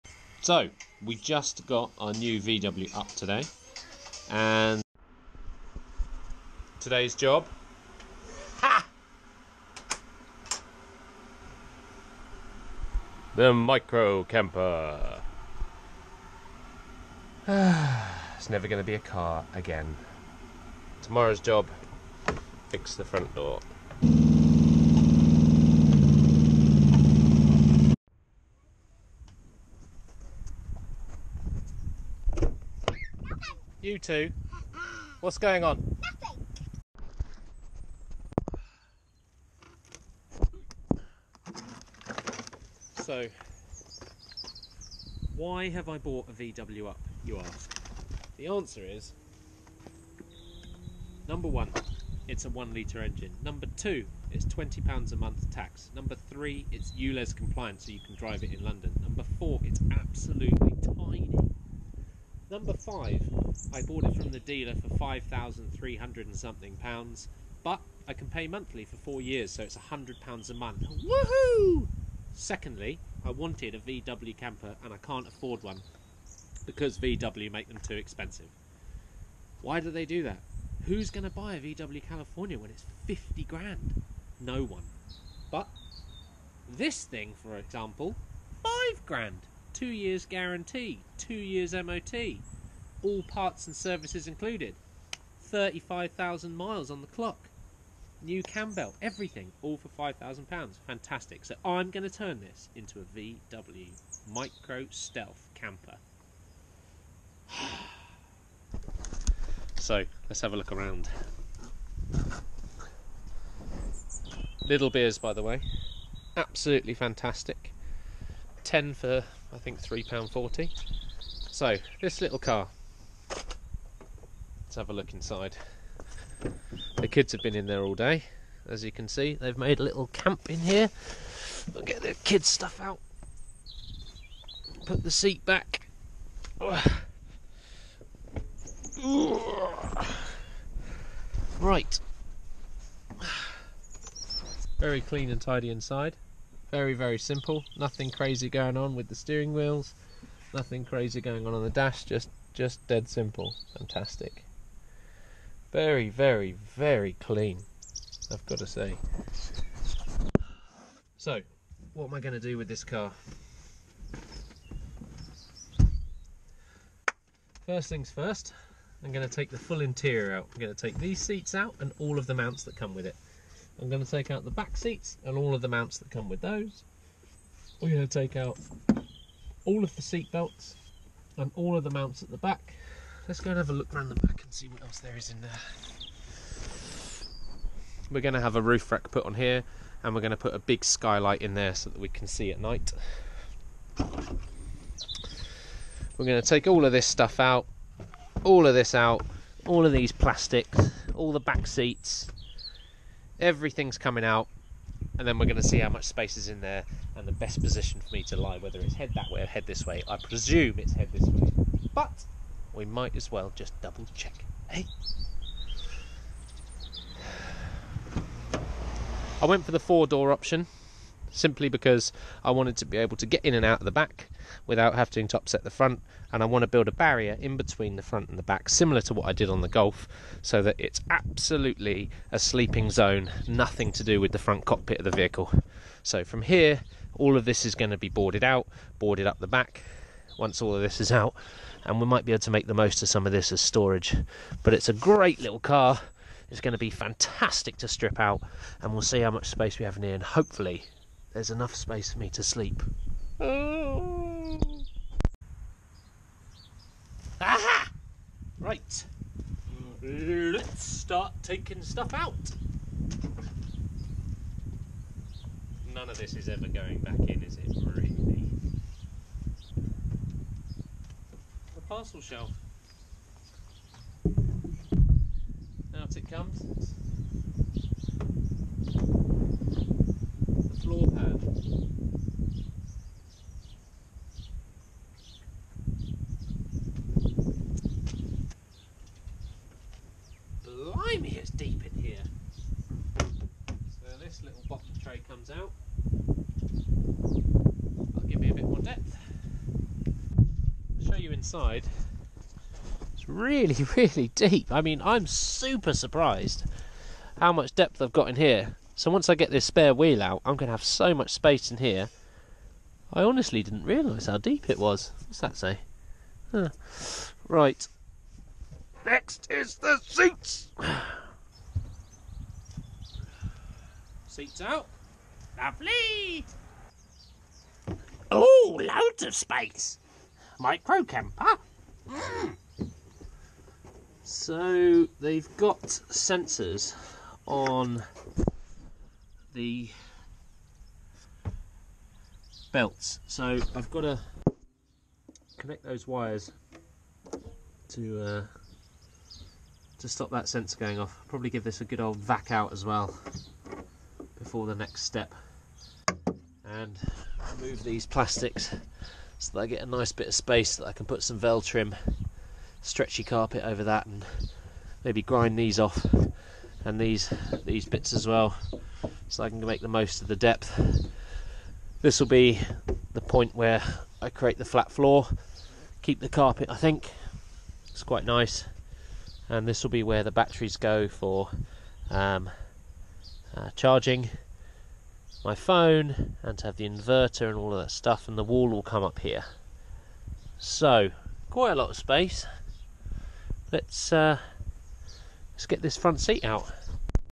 How to make a VW UP STEALTH CAMPER PRT 1
I want a vw camper, i need to live in london and be on stealth mode, so what better car to do it with than a vw up, cheapest smallest option available. watch these videos to see how it progresses